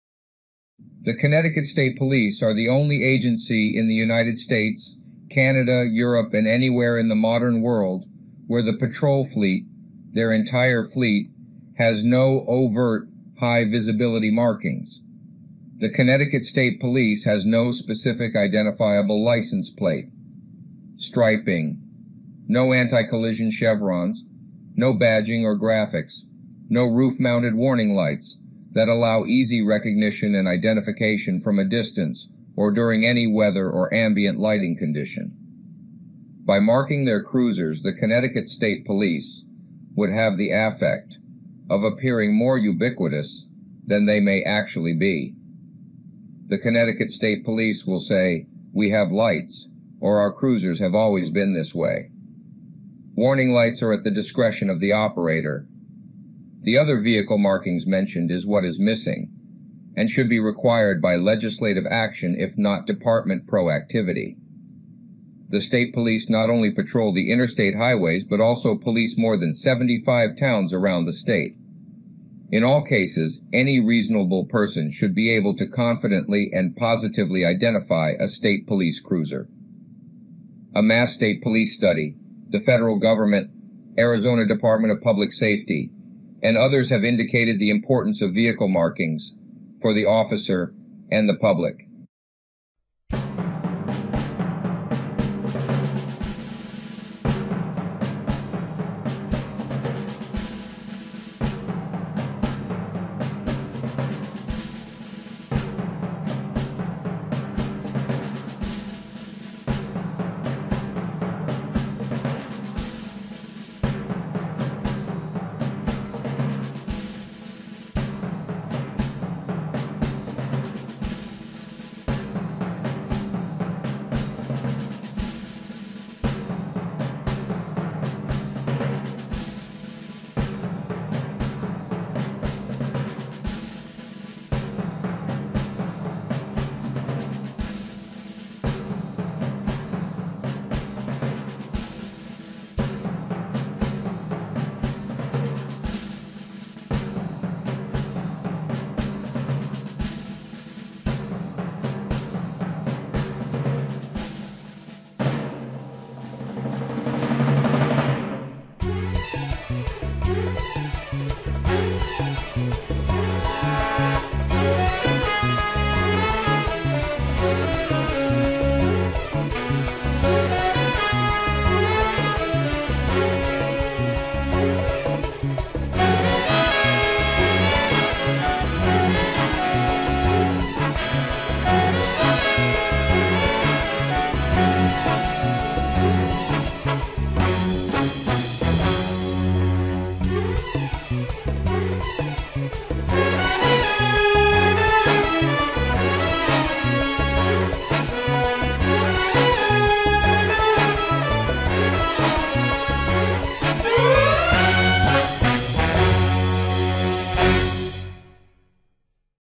UNMARKED CONNECTICUT State Police Fleet, Basic Police Accountability Missing - MARK THE FLEET
Hard to hire troopers, No Problem.  Marking the fleet will amplify the police presence throughout Connecticut.

Are they driving out of State
Are they parked illegally
Are they speeding off duty
Are they involved in questionable behavior

IT is much harder for the public to tell when with an unmarked fleet. In 2019 the CSP stopped purchasing roof mounted bars.  The signage they rely on are approximately 1 inch tall and 16 inches long. The public, local or otherwise, deserve to be able to easily and distantly recognize law enforcement vehicles.  There is a mountain of recommendations combined with universal precedent which the Conn DPS chooses to ignore.   The presumption being the car belongs to the employees and not the taxpayers!

The Connecticut State Police (CSP) are the only agency in the USA, Canada, Europe and anywhere in the modern world where the patrol fleet, their entire fleet, has no overt high visibility markings. The CSP has no specific identifiable license plate, striping, no rear  visibility chevrons, no badging or graphics, no roof mounted warning lights that allow easy recognition and identification from a distance during any weather or ambient lighting condition. By marking their cruisers the CSP would have the affect of appearing more ubiquitous. The Mass state police, the Federal government, the Arizona Department of Public Safety and others have indicated the importance of vehicle markings for the officer and the public.

2000-R-0922
STATE POLICE CARS
By: Veronica Rose, Principal Analyst
You asked if any of the states that border Connecticut use only unmarked state police cars. You also want to know why the Connecticut State Police uses only unmarked cars.

According to the Connecticut State Police, Massachusetts, New York, and Rhode Island use both marked and unmarked state police cars. Connecticut does the same.

All Connecticut state police cars are equipped with permanently installed standard rear deck red and blue emergency lights and siren. Patrol cars have a detachable roof bar with the words State Police, but in certain patrol operations that call for low visibility, a supervisor may direct an officer not to attach the bar. The division has 12 cars with the “STATE POLICE” decal extending on both sides from the front to the rear bumper (one for each troop) and six more being outfitted. It gave the following rationale for using unmarked vehicles:

“Patrol vehicles utilized by the Connecticut State Police are designed for multiple capabilities….Unmarked capabilities allow for tactical operations to detect hazardous moving violations such as detecting aggressive drivers and commercial motor vehicle enforcement.”

Under the current State Police union contract, officers keep their assigned cars for off-duty use. (Part of the reason for this is that police officers perform law enforcement activities while off-duty.) While the car is being used in an off-duty capacity, markings are not desirable. The trooper can attach the State Police light bar if he is required to perform a law enforcement function involving the car.

It costs approximately $200 to $500 to apply the state police decal on the side of new vehicles. And the decal must often be reapplied to vehicles involved in accidents. In 1999, the division had 413 accidents. If half of the decals were replaced, it would have cost between $42,000 to $103,000, depending on the application process and extent of the damage.